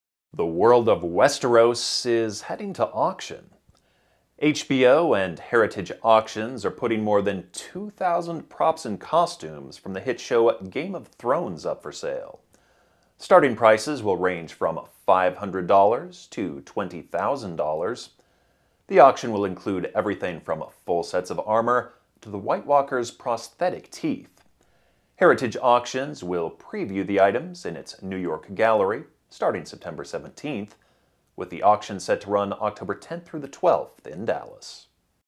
Game of Thrones Props and Costumes to Auction
The world of Westeros is heading to auction. HBO and Heritage Auctions are putting more than 2,000 props and costumes from the hit show "Game of Thrones" up for sale. Starting prices will range from $500 to $20,000.